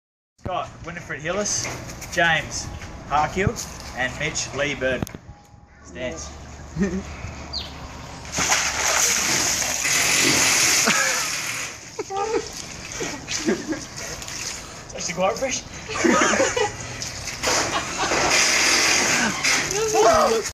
CJ Ice bucket
Mr mermaid smashing the ice bucket challenge.